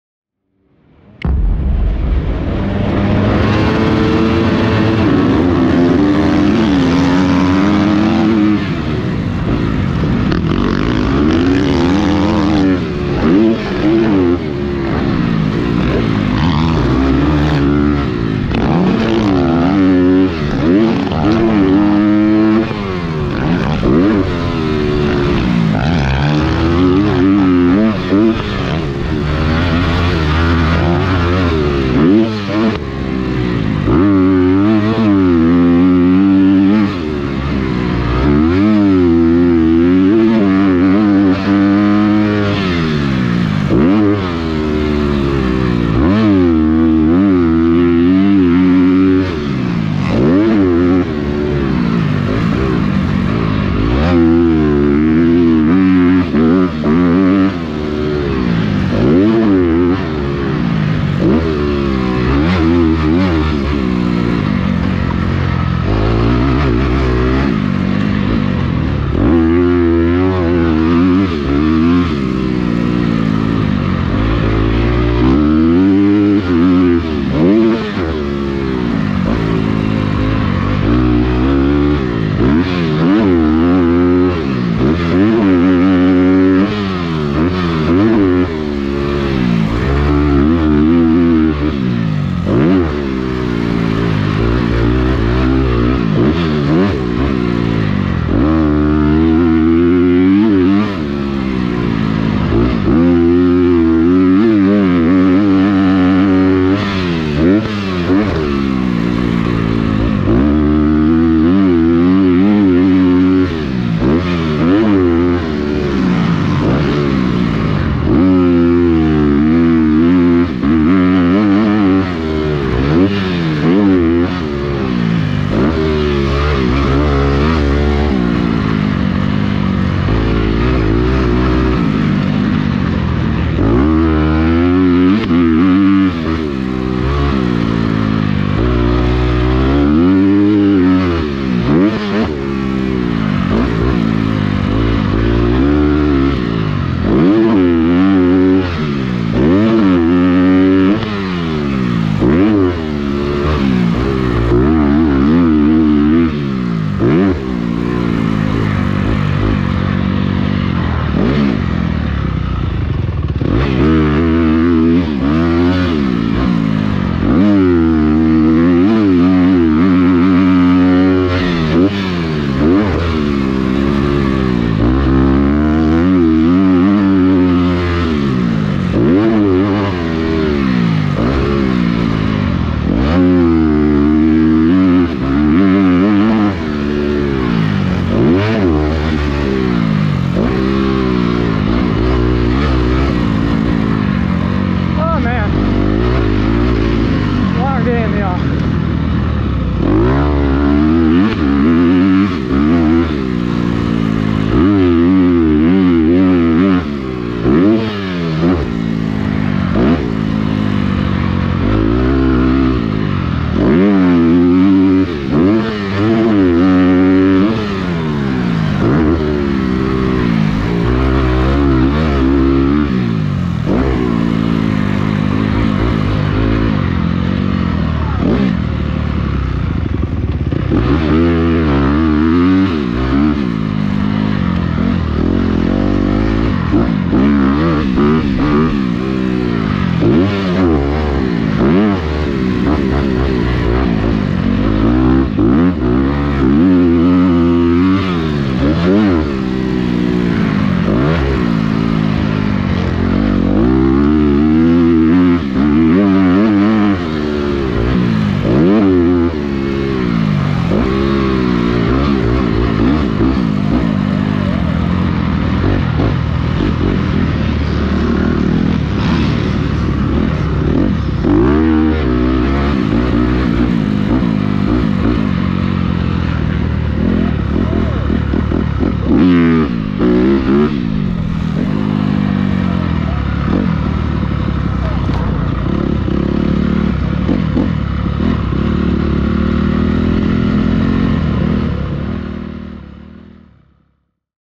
2020 Morelands Motocross Under The Lights Rd 3 | 14PLUS Moto 2
Was able to see what my new bike was capable of at the third round of the Under The Lights Series at Morelands, here's my on-board from 14+ moto 2!